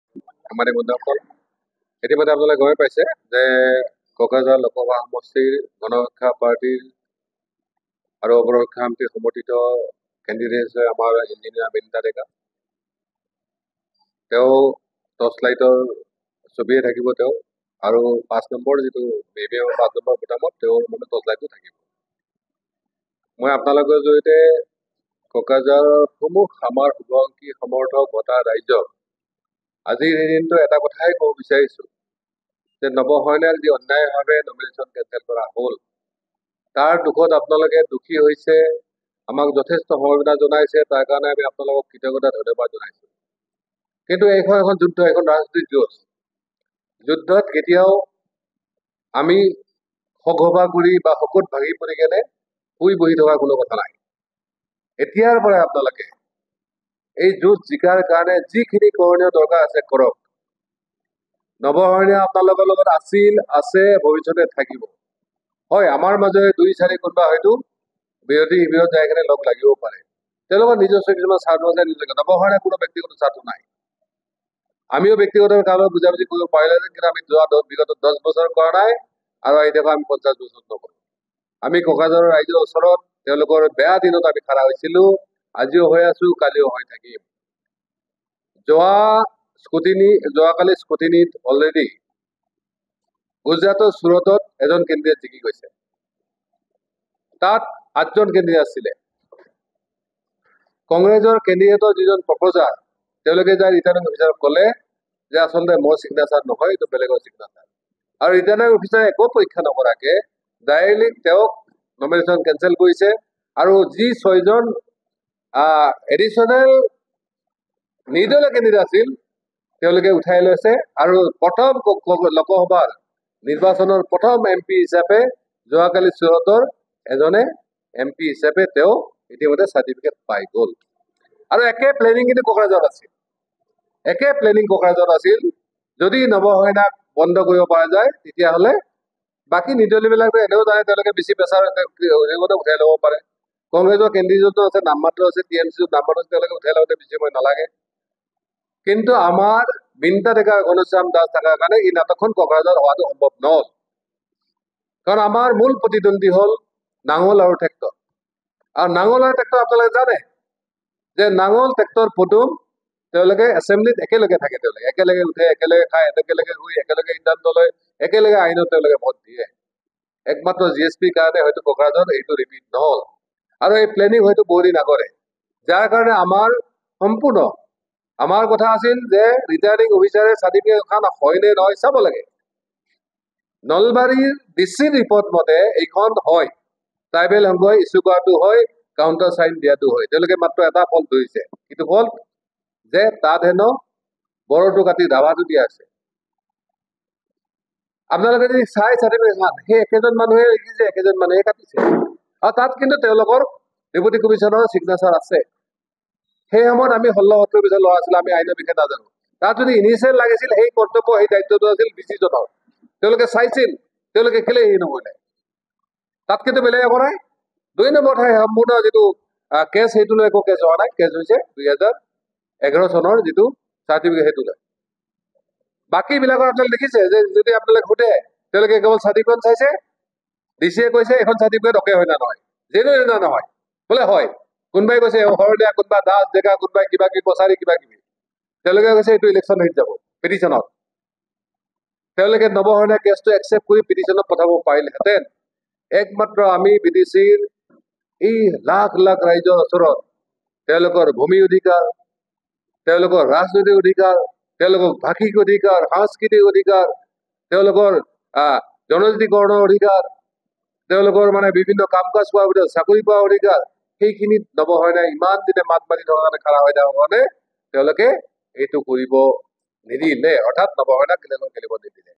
Naba Sarania Pressmeet |'আমি ভয় কৰা মানুহ নহয়, আমি কামাণ্ডাৰ মানুহ।' সংবাদমেলত নৱ শৰণীয়াৰ মন্তব্য
Naba Sarania Pressmeet |'আমি ভয় কৰা মানুহ নহয়, আমি কামাণ্ডাৰ মানুহ।' সংবাদমেলত নৱ শৰণীয়াৰ মন্তব্য। N18V